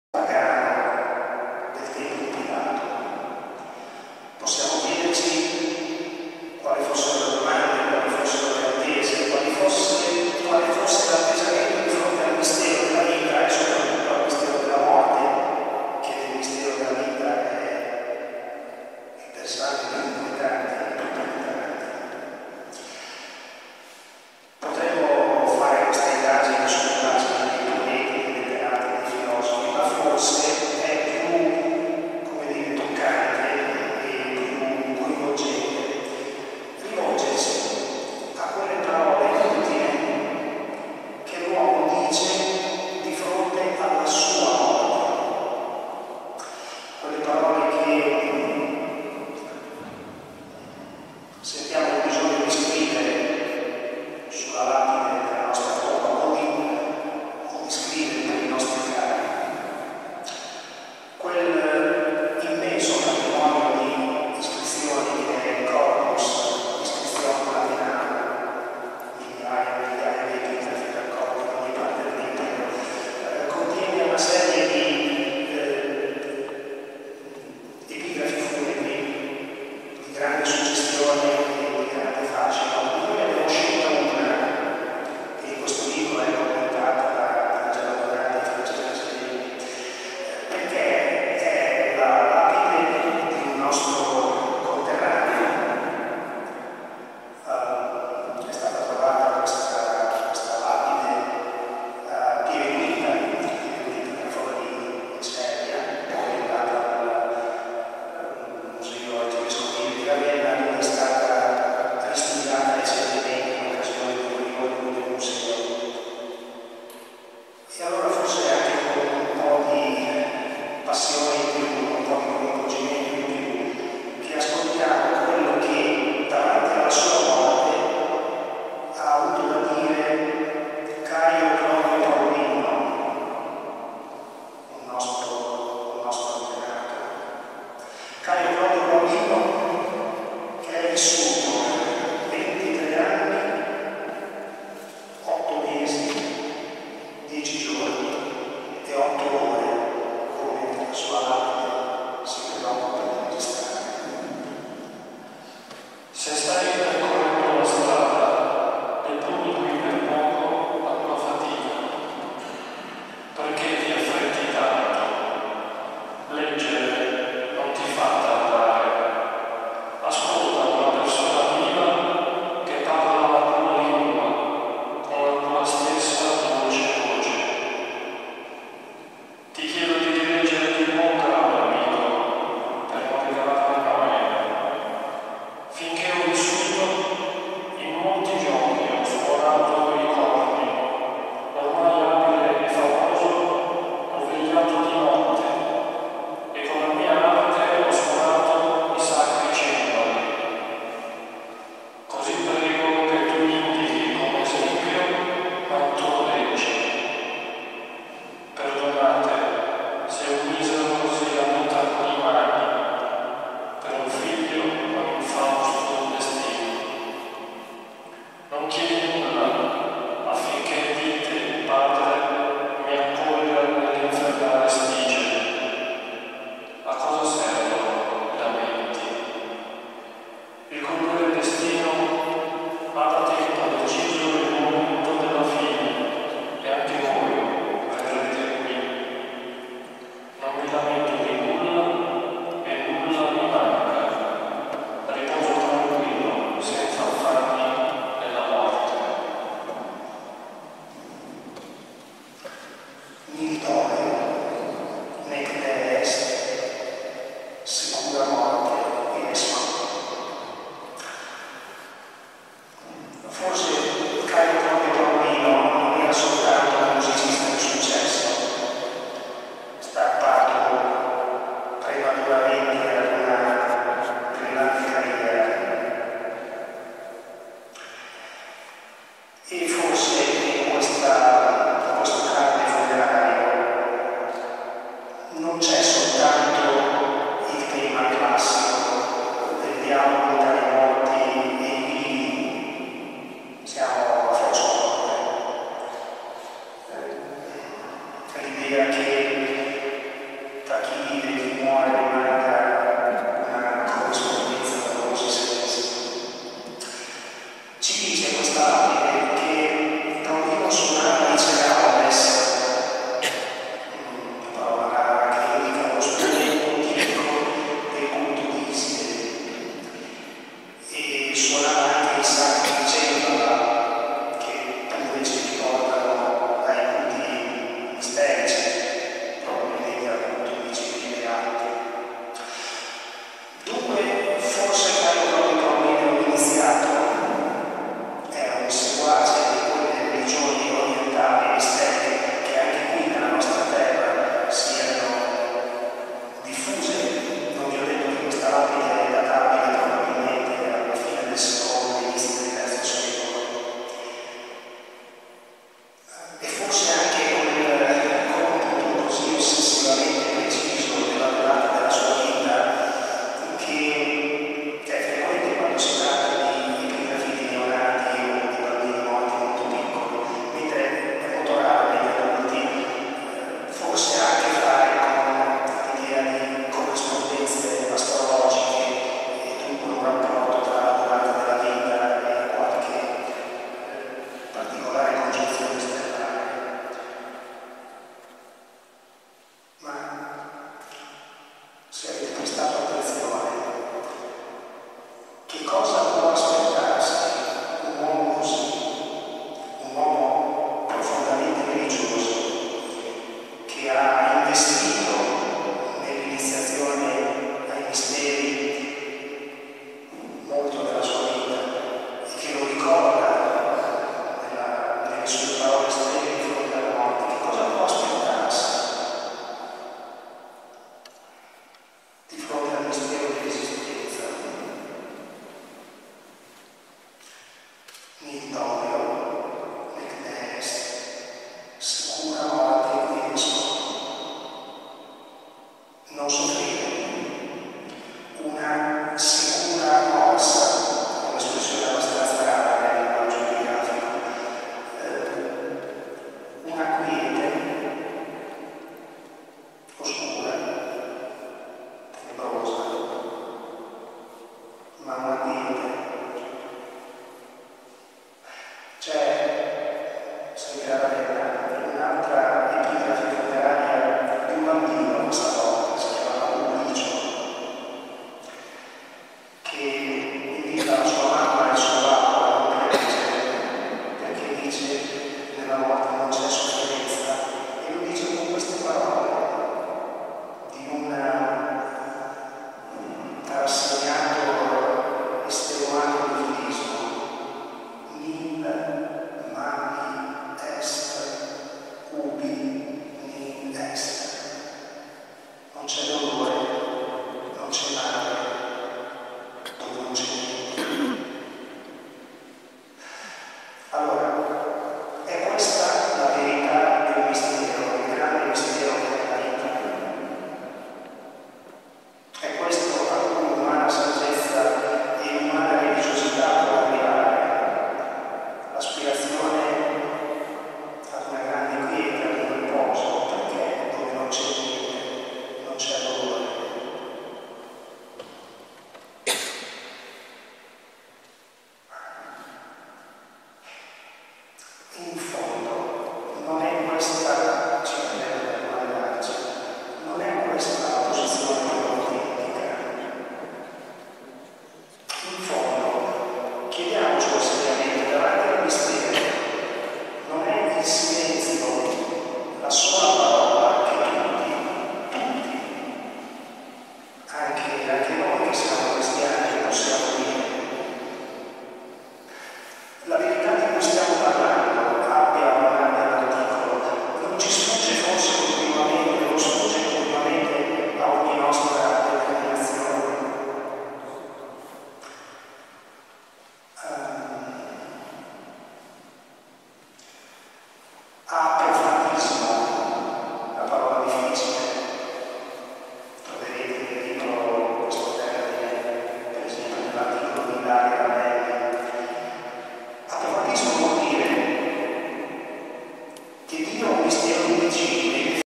parte 3/8 serata di lettura testi Mysterium simplicitatis
Le riprese della serata di letture di testi dell'antichità cristiana e pagana "Mysterium simplicitatis. Verità e mistero nel pluralismo culturale della tarda antichità" proposta dal Centro culturale "Campo della Stella" di Cesena giovedì 13 maggio 2010, alle ore 21, nella Basilica di Santa Maria del Monte.